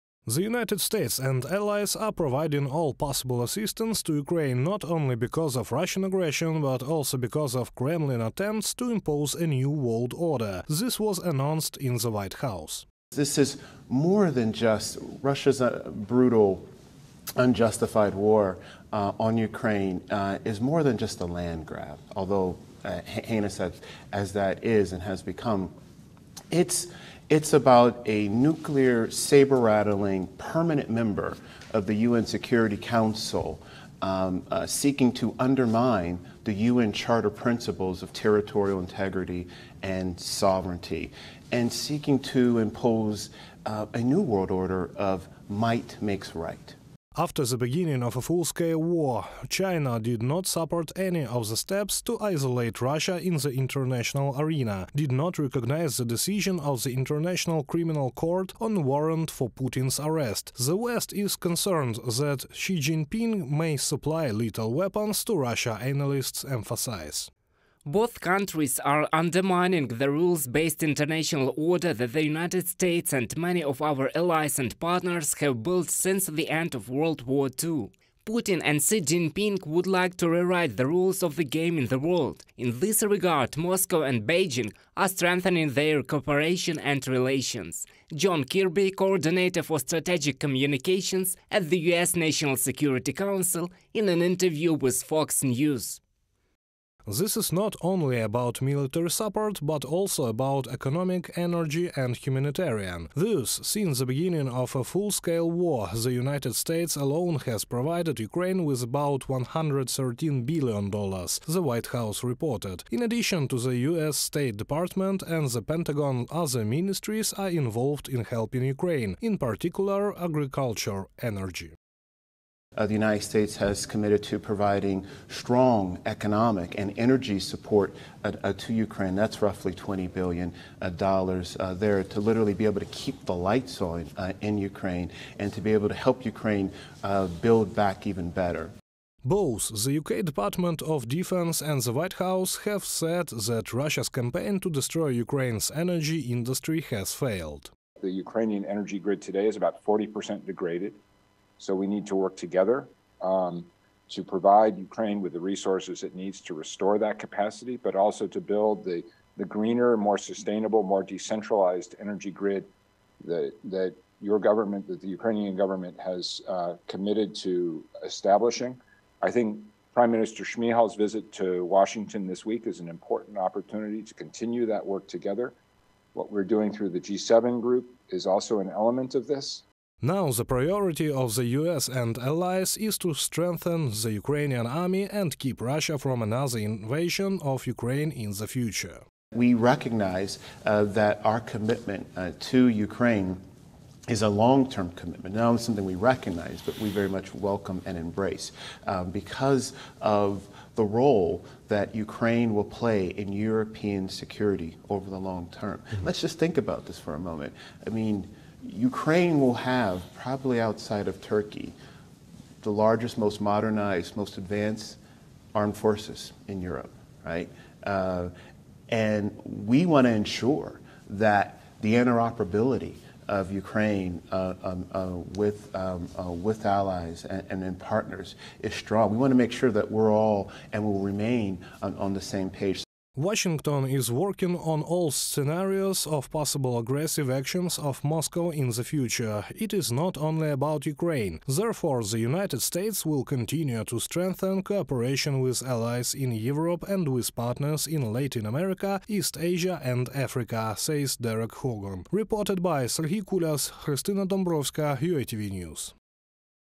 Autocracy, dictatorship, right of the strong: Russia has power over its citizens, not over the world
Russia seeks to create chaos not only in Ukraine but also in the world. This was announced by the US State Department. Washington is concerned about military interaction between Beijing and Moscow. Autocracy, dictatorship, right of the strong - what way Kremlin is heading? Details - in our story.